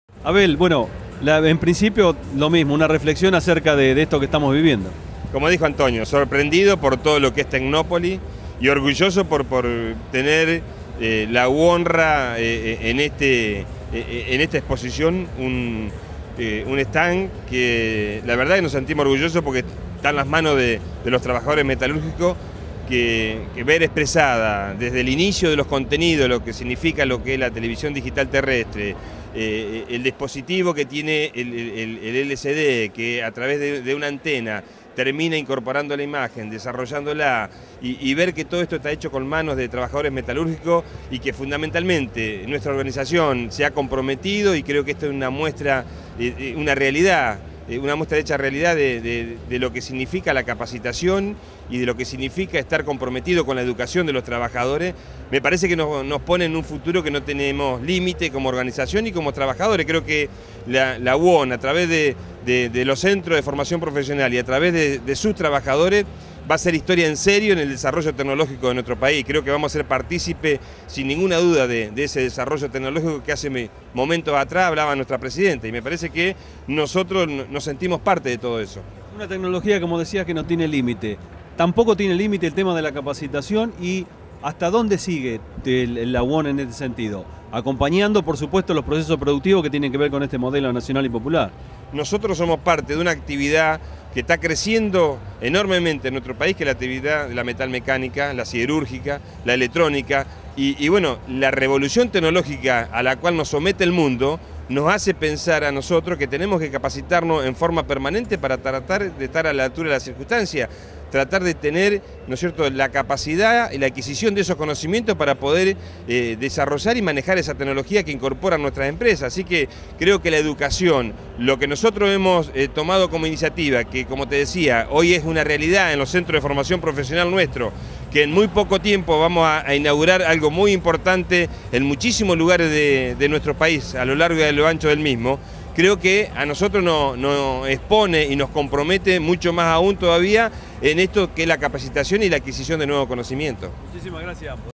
La UOM en TECNOPOLIS
Abel Furlán comenta la participación de la Unión Obrera Metalúrgica en el stand de la Televisión Digital Terrestre